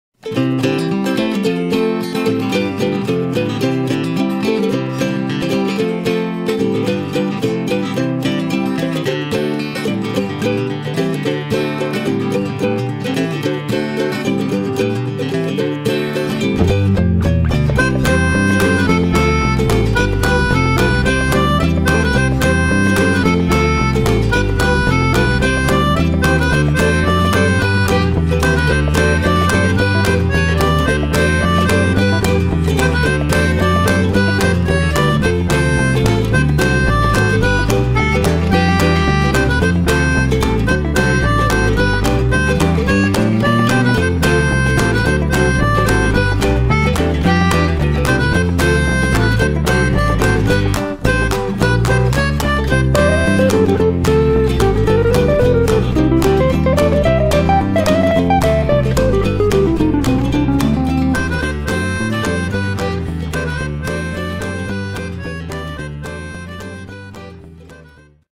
Stove Top Bubble and Squeak
Classic bubble and squeak is usually a mix of cabbage and mashed potato, often topped with cheese and then heated to bubbling in the oven.  This version is chunkier, heated in a pan and made golden under the grill